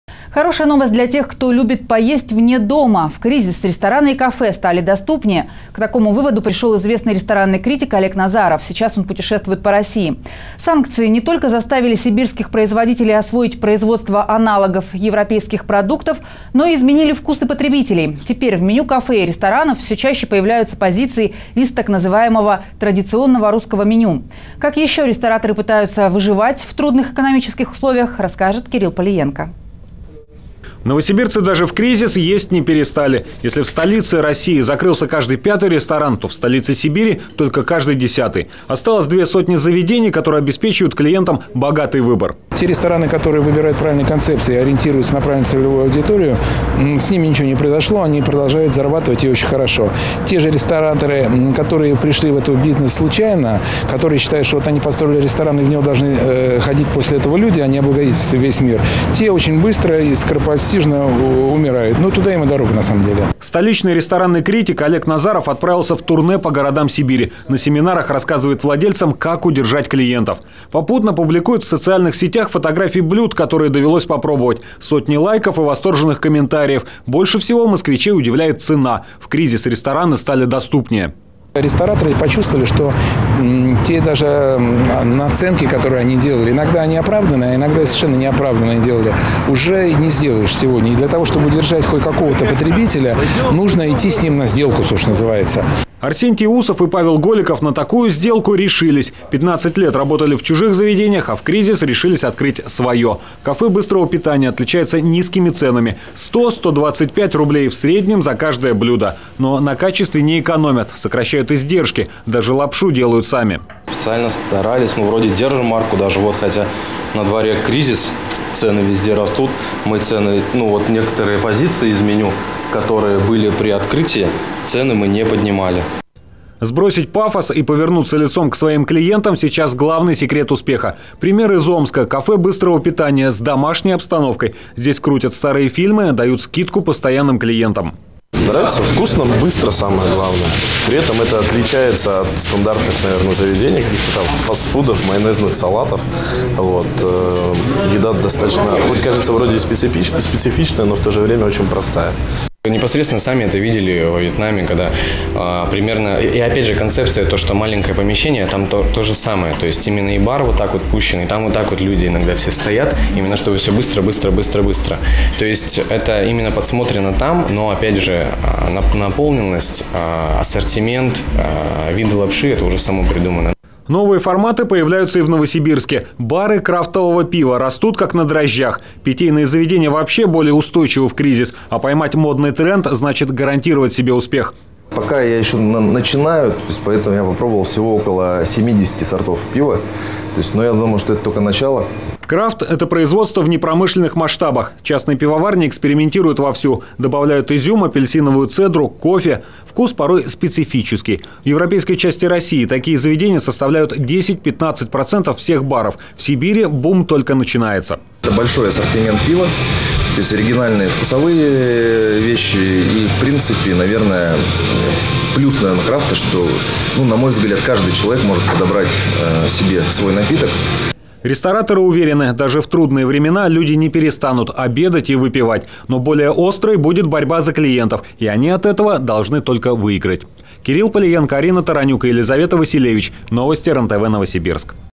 Сюжет от 02.10.2015 рестораны в кризис
Оригинальные блюда, напитки и расцвет крафт-баров. на что рестораторы делают ставки во время кризиса?